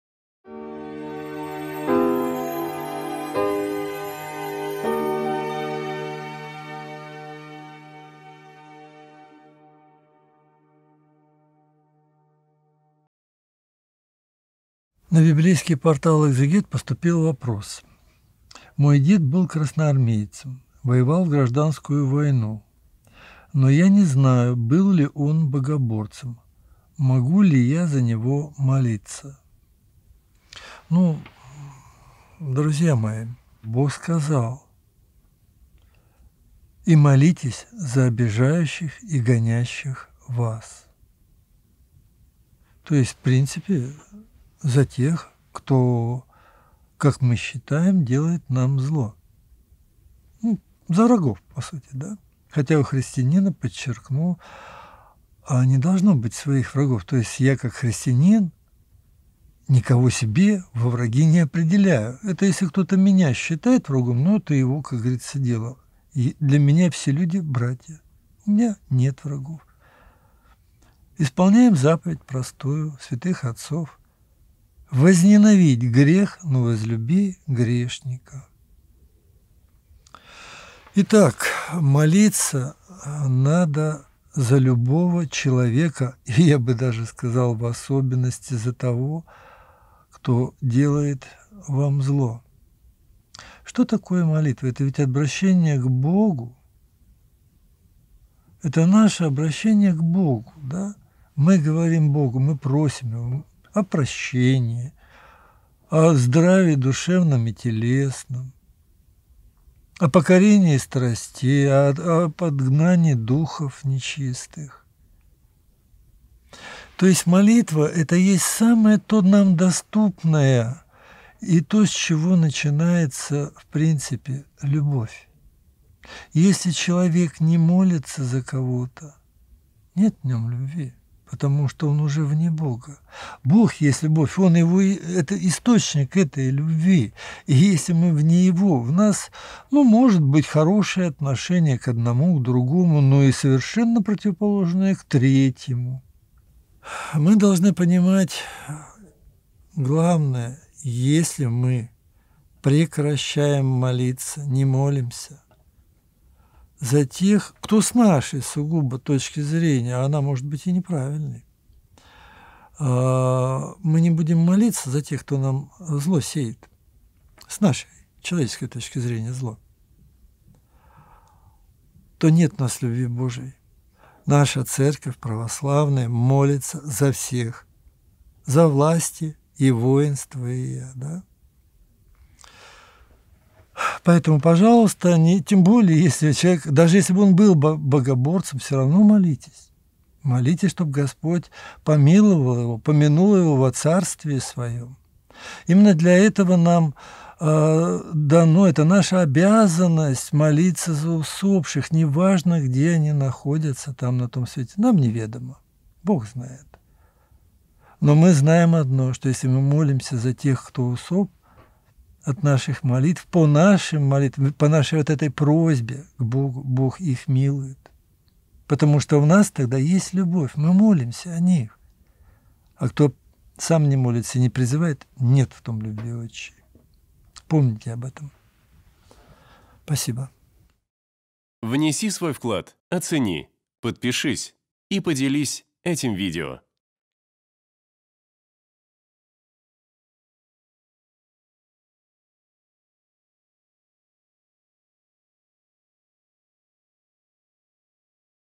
Дед был красноармейцем. МОЖНО ЛИ за него МОЛИТЬСЯ ? Инок Киприан (Бурков)
В сегодняшнем видео на портале "Экзегет" инок Киприан (Бурков)  рассуждаем на тему: "Дед был красноармейцем. Можно ли за него молиться".

🔸 Молитва за врагов.
🔸 Покорение страстей.
🔸 Проявление христианской любви.
🔸 Поминовение усопших.
🔸 Призыв к молитве.